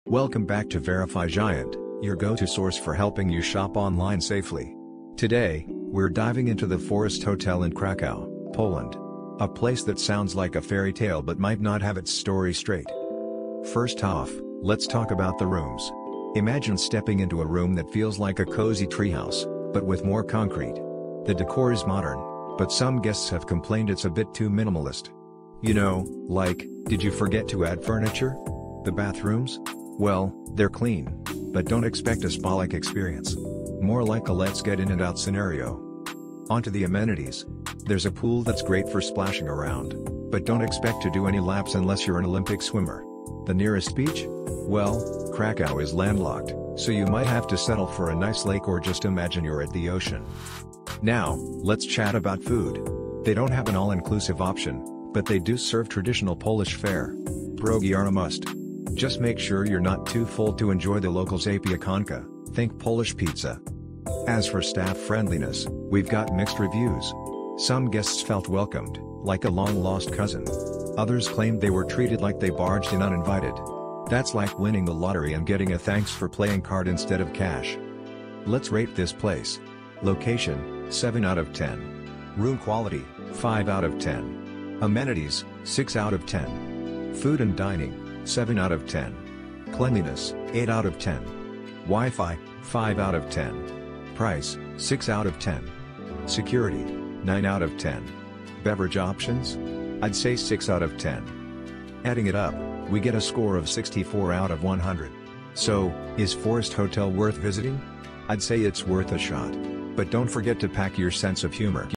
Forest Hotel, Kraków, Poland
Discover the enchanting Forest Hotel in Kraków, Poland, your ultimate destination for an unforgettable stay! Nestled amidst lush greenery, this eco-friendly hotel offers a unique blend of luxury and nature, making it one of the best hotels in Kraków for travelers seeking relaxation and adventure. In this video, we explore the stunning accommodations available at Forest Hotel, perfect for families